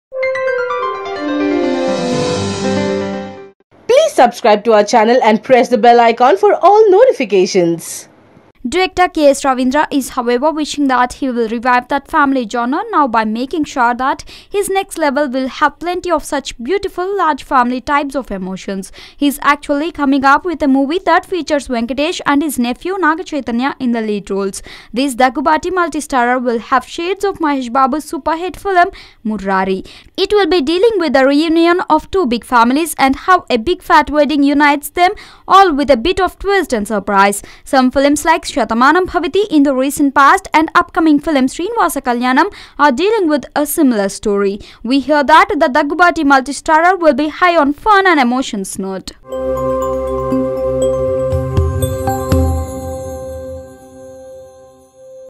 Is Daggubati Heroe’s Are Planning To Remake Mahesh Babu’s Movie..? | Tollywood News | YOYO Times
Is Daggubati Heroe’s Are Planning To Remake Mahesh Babu’s Movie..?
Director KS Ravindra (Bobby) is, however, wishing that he will revive that family genre now by making sure that his next will have plenty of such beautiful large-family types of emotions. He's actually coming up with a movie that features Venkatesh and his nephew Naga Chaitanya in the leads. this Daggubati multi-starrer will have shades of Mahesh Babu's super hit film "Murari". It will be dealing with the reunion of two big families and how a big fat wedding unites them all with a bit of twist and surprise. some films like Satamanam Bhavati in the recent past and upcoming film Srinivasa Kalyanam are dealing with a similar story, we hear that the Daggubati multi-starrer will be high on fun and emotions note.